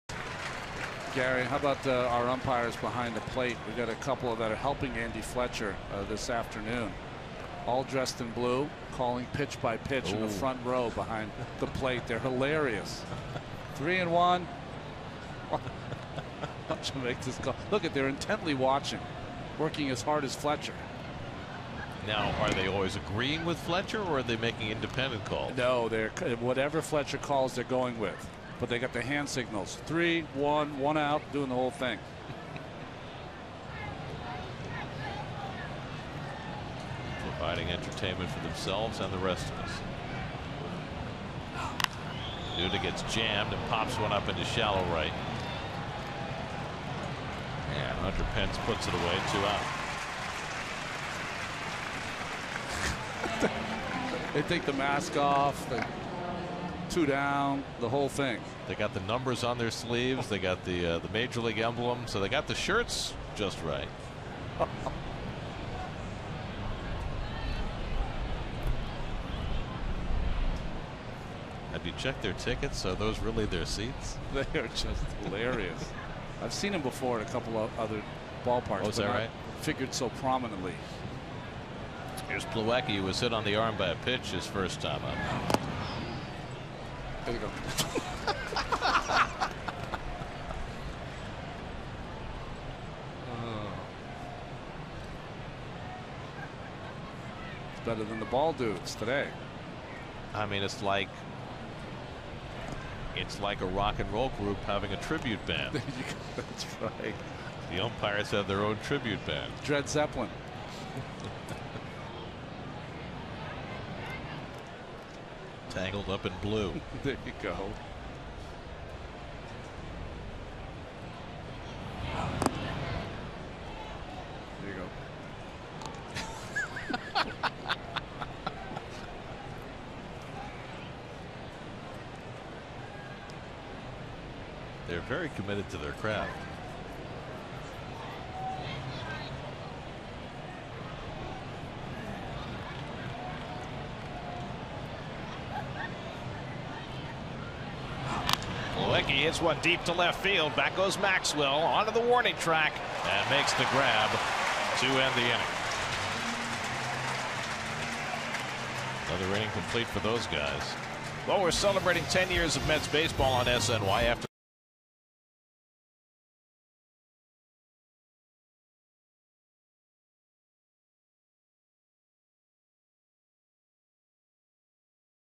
A pair of umpires call game from stands
7/8/15: A pair of fans dressed as umpires call the Mets-Giants game from the stands